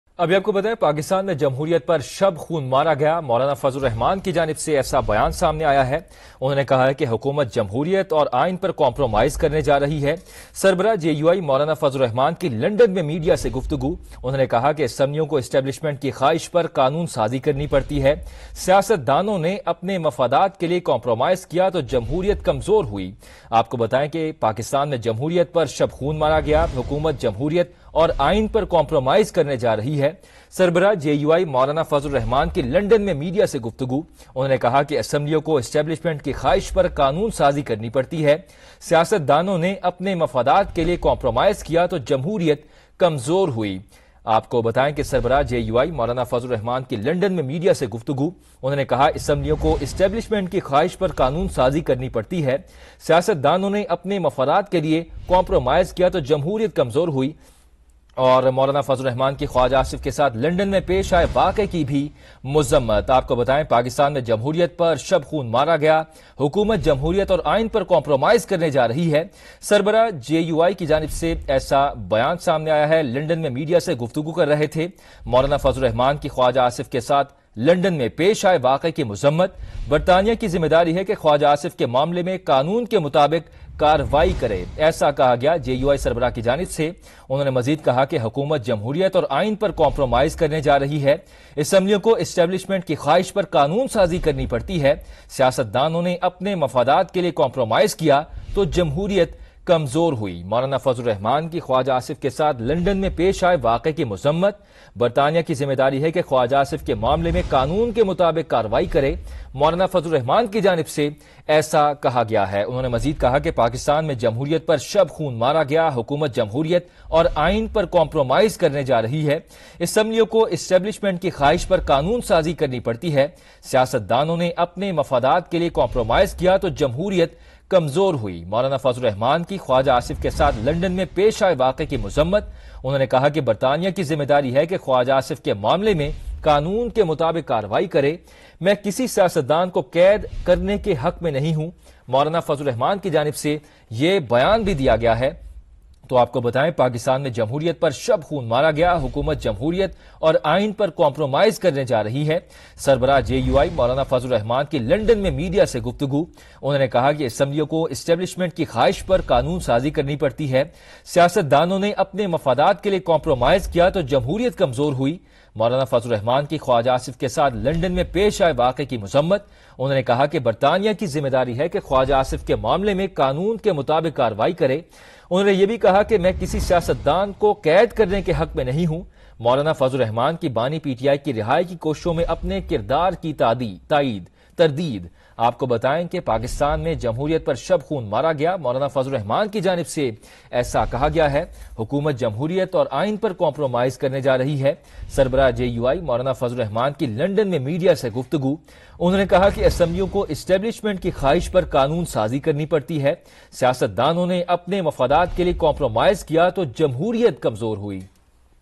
Maulana Fazal ur Rehman's Important News Conference In London | SAMAA TV
Attack on Former Chief Justice Qazi Faez Isa's Car in London | Latest Update | SAMAA TV